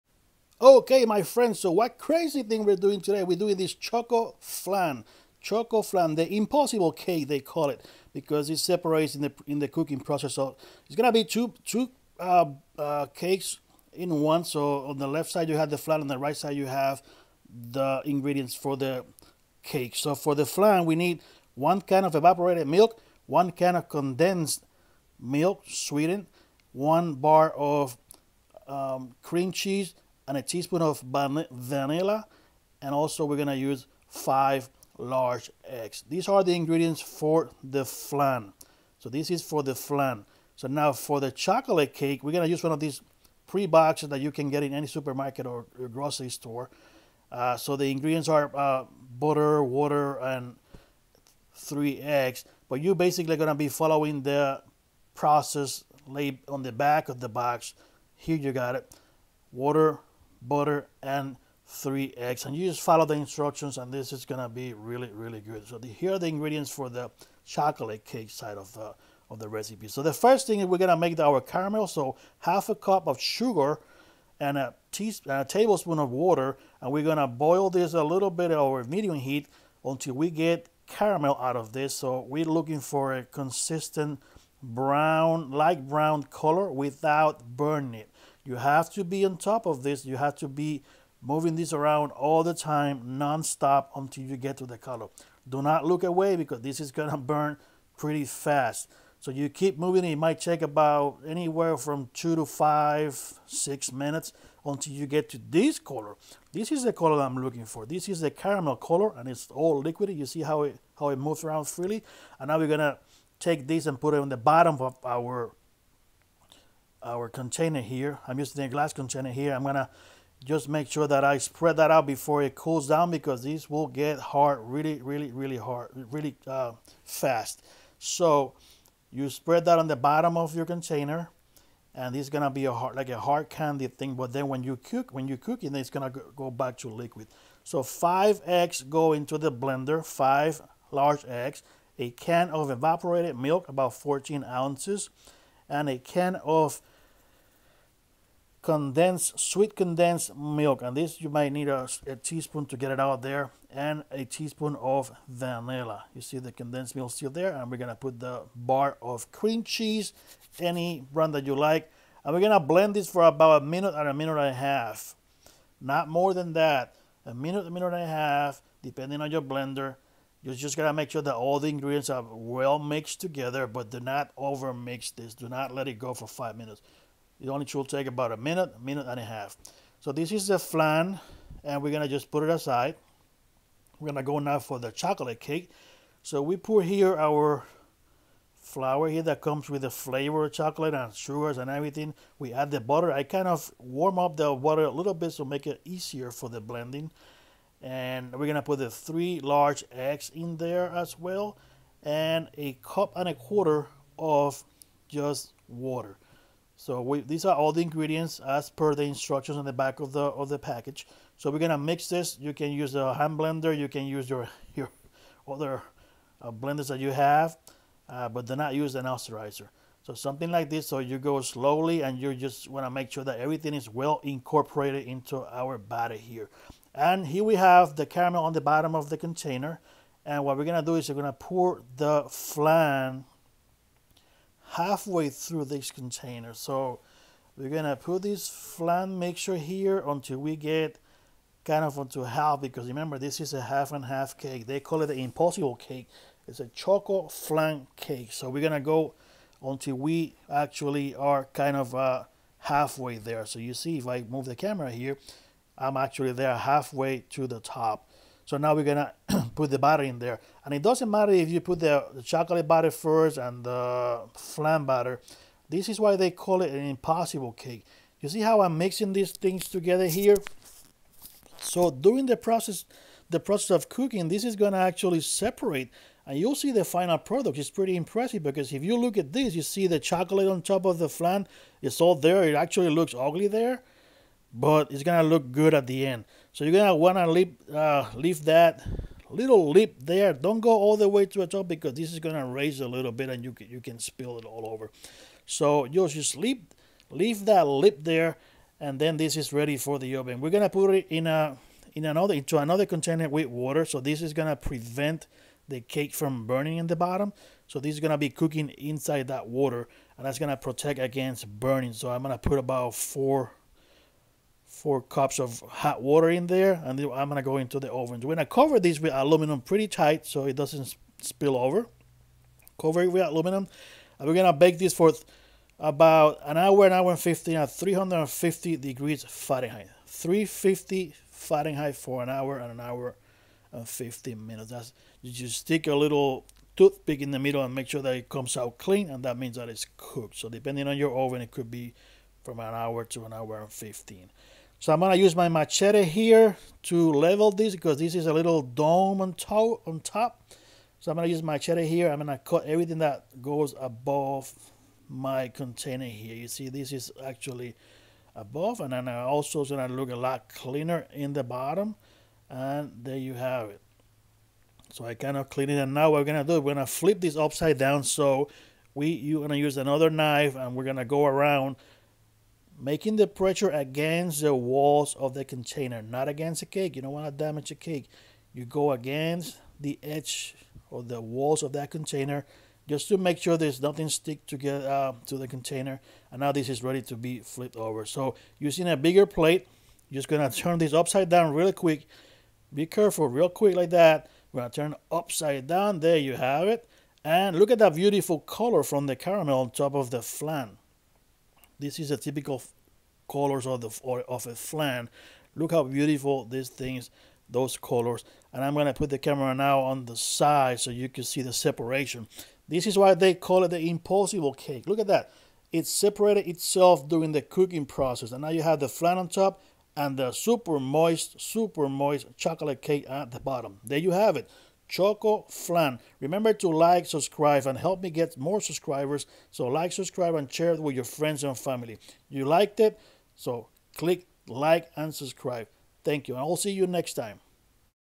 The Impossible Cake | ChocoFlan | You won’t believe it
This cake is called the Impossible Cake; but today I show you how to do it!

Check all my videos ! / Chequea todos mis videos !